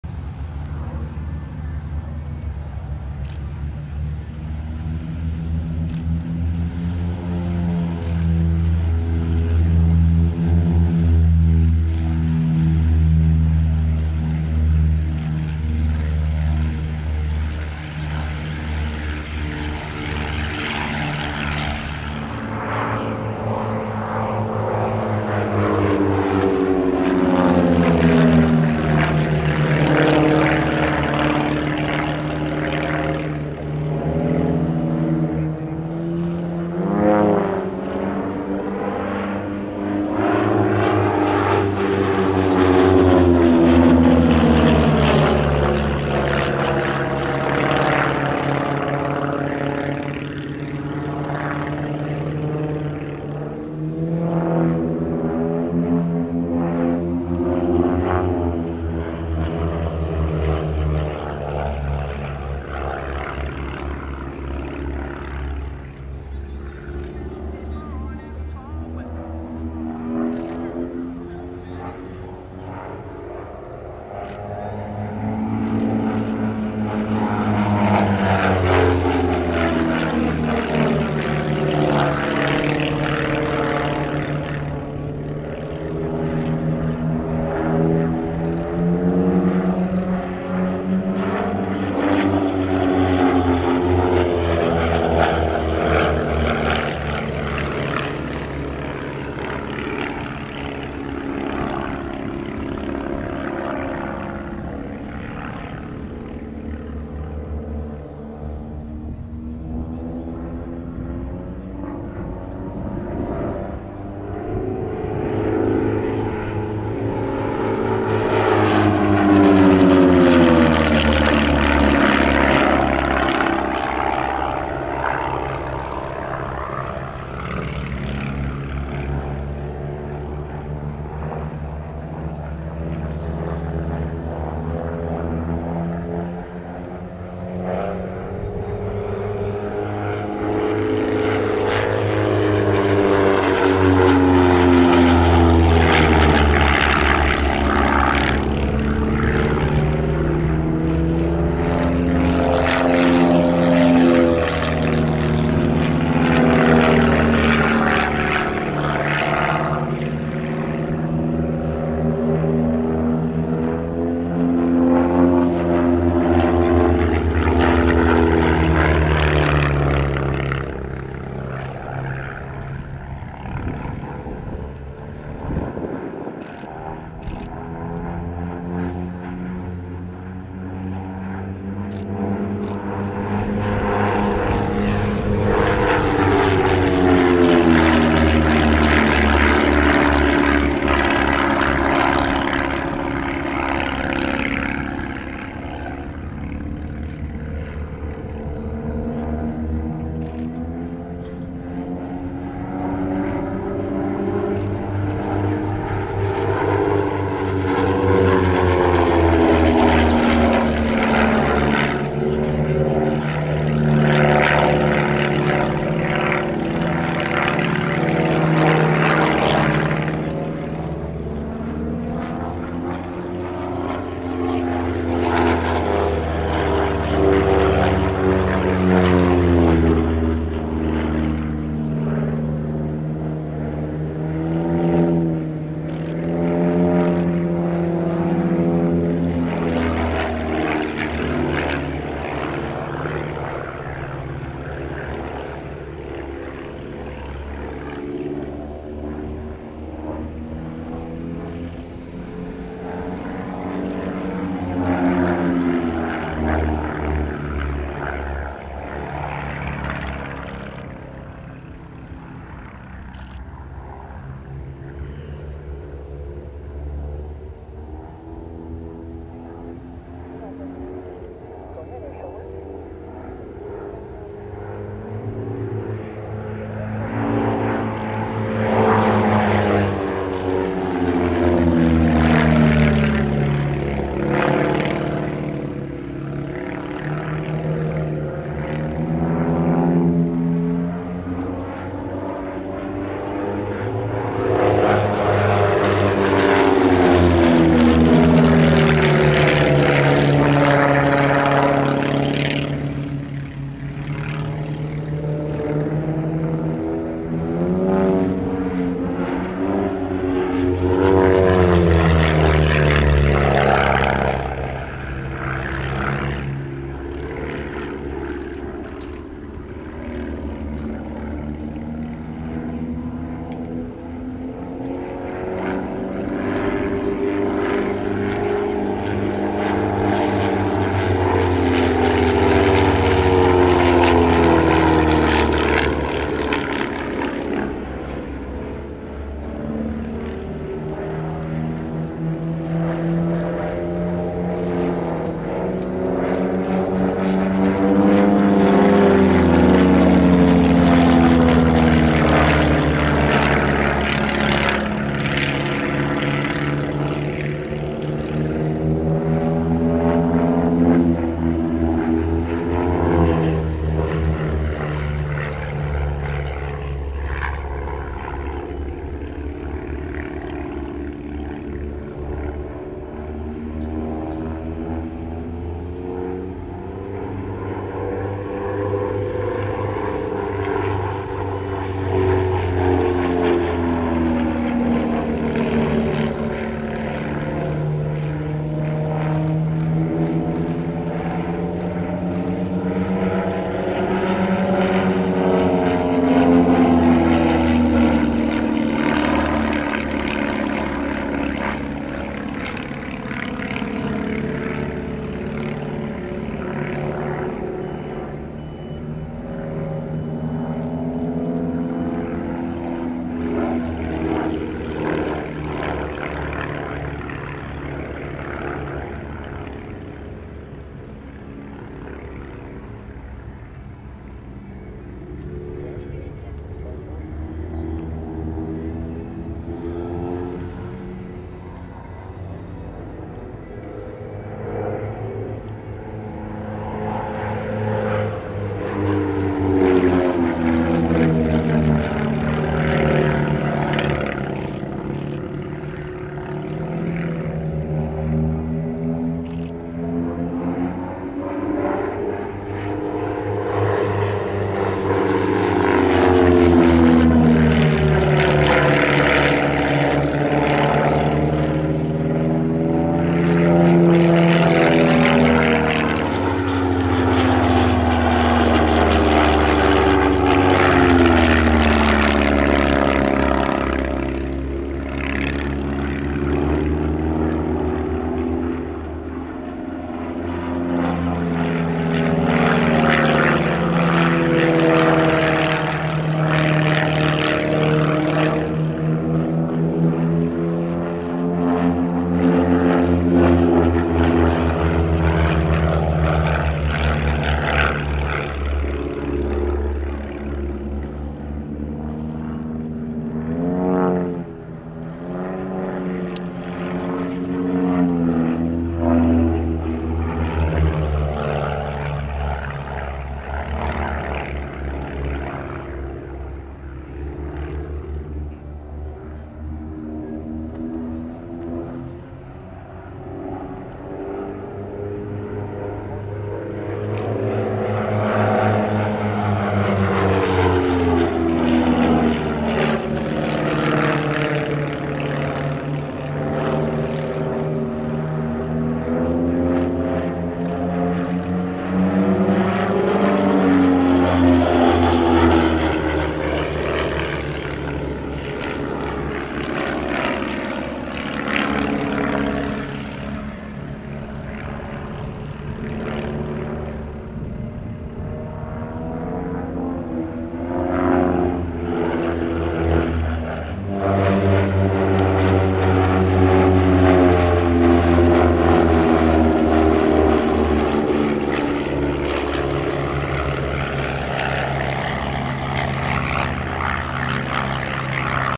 Undaunted Airshows - Alberta International Airshow 2023
Undaunted Airshows is a relatively new airshow act, involving 2 Van's Aircraft (RV-7 and RV-8), flying in close formation. Stephen Christopher and Todd Rudberg are two of the nicest pilots I've met. Their act is unique and engaging. I loved the opportunity to film them, and then talk with them about their experiences as pilots.

Looking forward to crossing paths again!